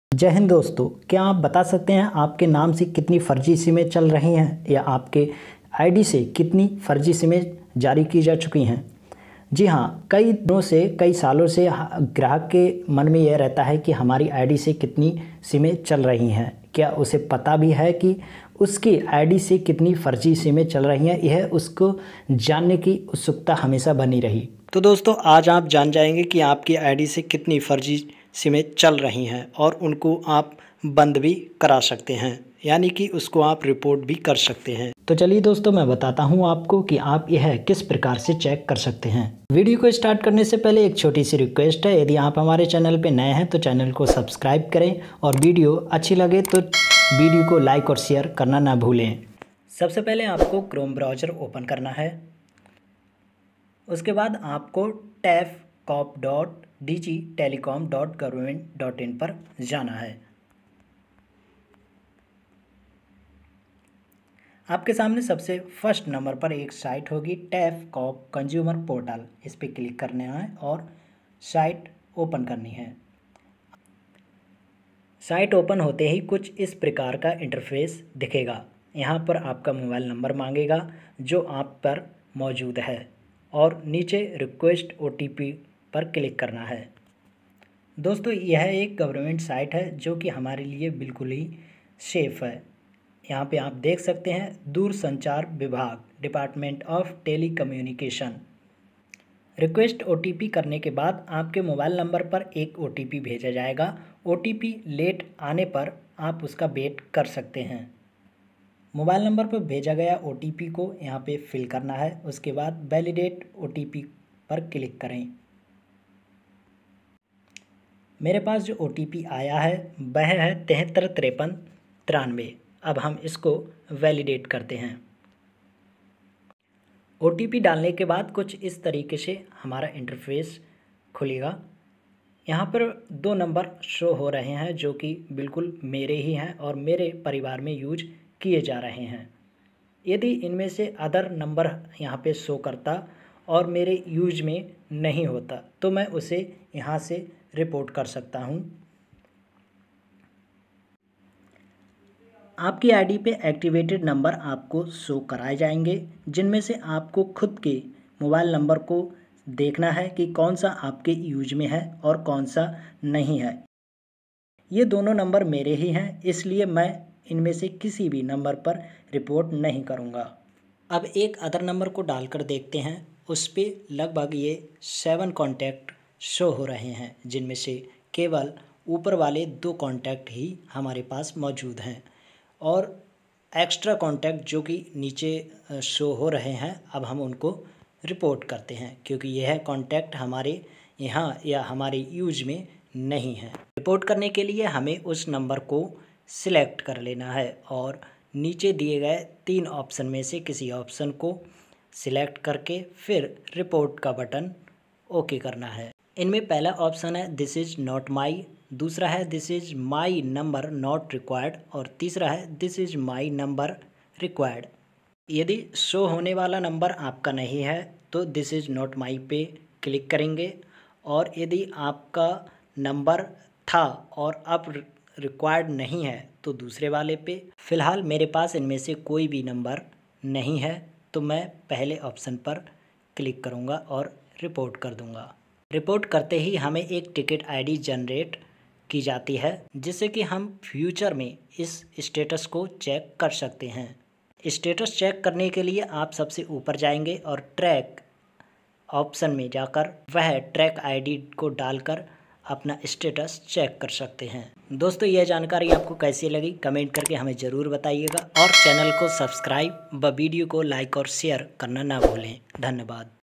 आपकी आईडी से कितनी फर्जी सिम चल रहे है जल्दी पता करें ।आधार कार्ड पर कितने सिम चल रहे हैं
दोस्तों आए दिनों में भ्रष्टाचार बढ़ता ही जा रहा है और हम जब भी कोई नया सिम एक्टिवेट कराते हैं इस समय भ्रष्ट रिटेलर मिल जाने के कारण
TAF-COP_Portal
The Telecom Analytics for Fraud management and COnsumer Protection